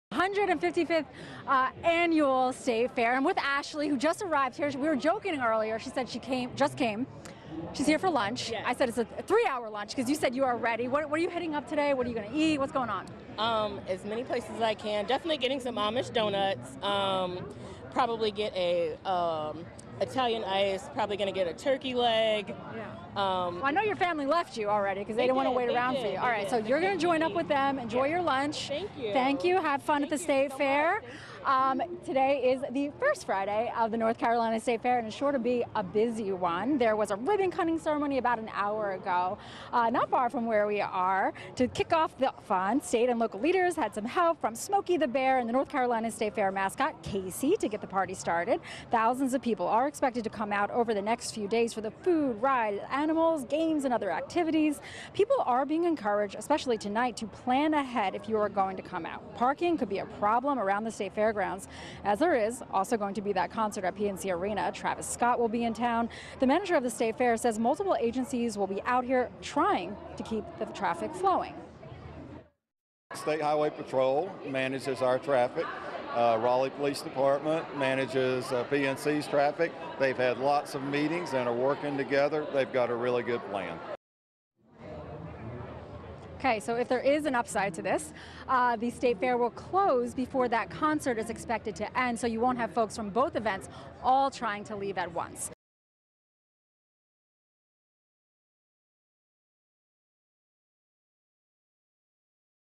Let the fun begin! The 2023 NC State Fair officially starts
The fair this year will have something for everyone including two new entertainment acts, four new rides, animals, family activities, and more than 50 new food items.